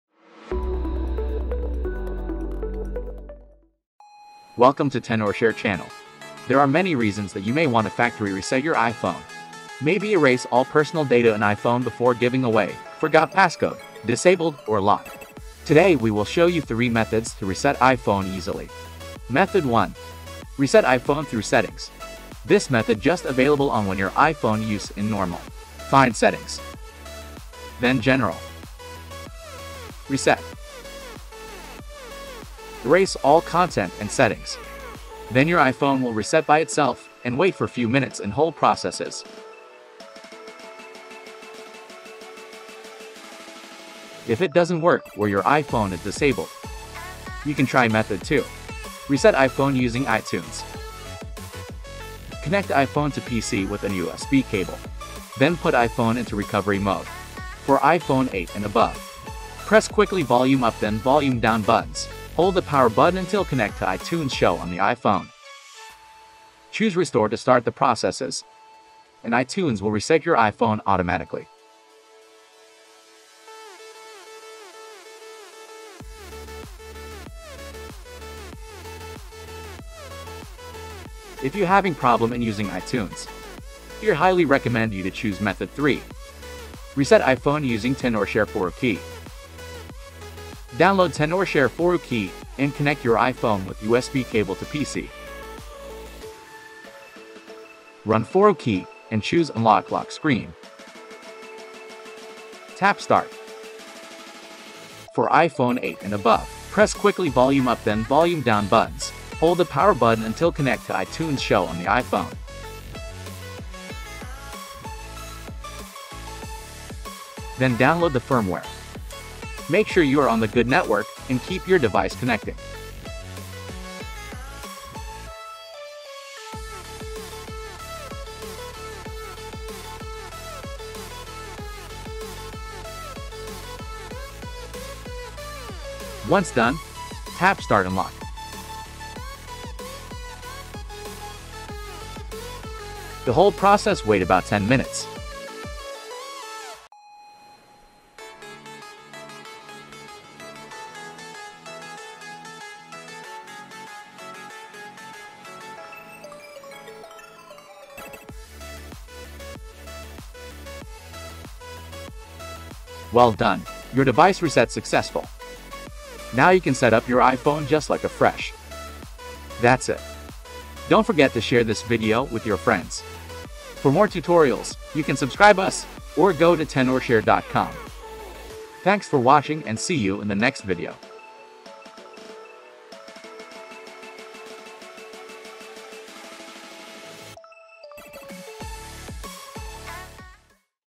How to Reset iPhone
There are many reasons that you may want to factory reset your iPhone. Maybe erase all personal data in iPhone before giving away、forgot passcode、disabled or locked. Today we will show you three methods to reset iPhone easily.

⚠Important notice:
2. 4uKey will erase all content and settings from the device just like iTunes.
3. You can restore your device from a backup after the unlocking if you have backups stored in iCloud or your computer.
4. 4uKey is unable to obtain access to credentials, compromise personal data or cause serious harm to others. Do not try to violate YouTube community guidelines.

Timestamp
00:00 Intro and preview
00:20 Reset iPhone through Setting
00:49 Reset iPhone using itunes
01:27 Reset iPhone using Tenorshare 4uKey